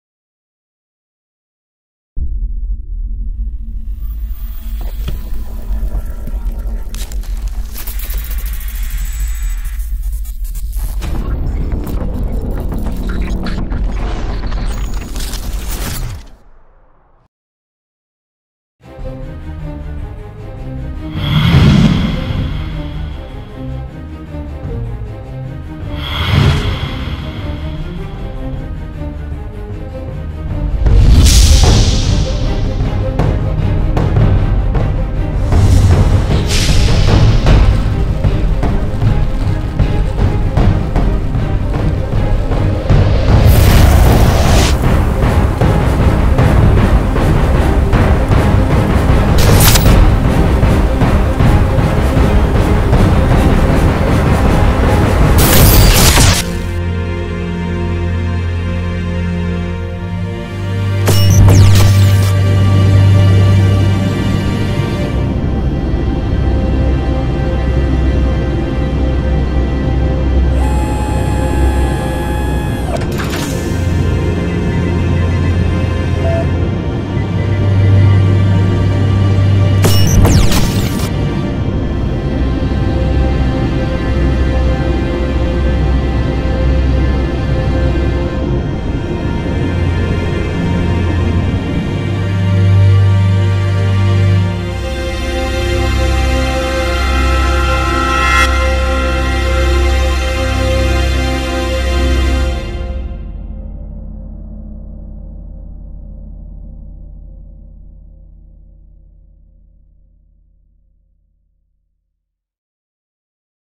Commercial Airliner Video Showcase
Video showcase of MLP SG, demonstrating the powerful ability to 3D animate with static images. Graphic scenes of extreme weather changes and flying scenes of a 747 Jumbo Jet.